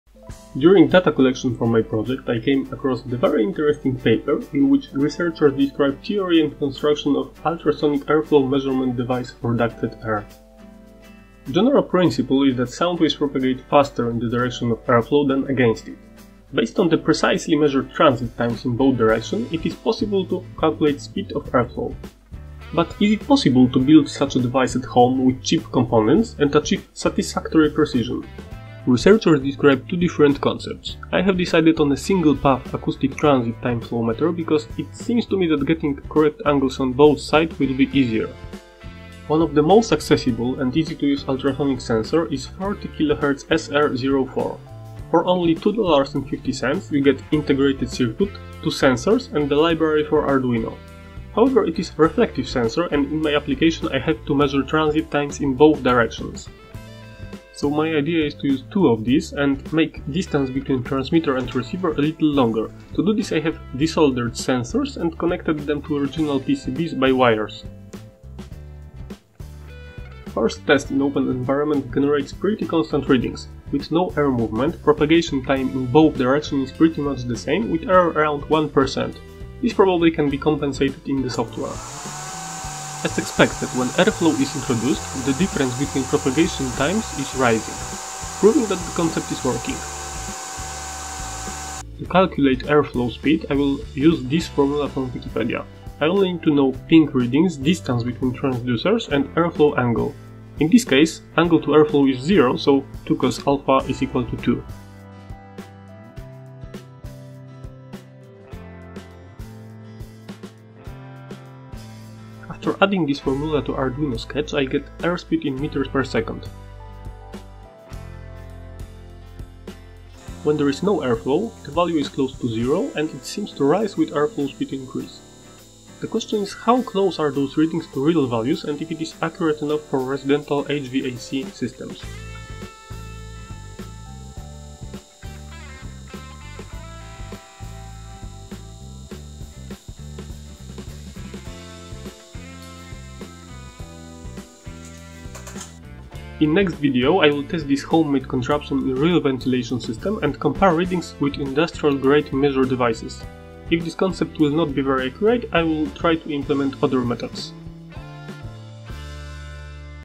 Building homemade ultrasonic air flow measurement device.
My attempt at building homemade ultrasonic air flow measurement device in HVAC ducts. Sources: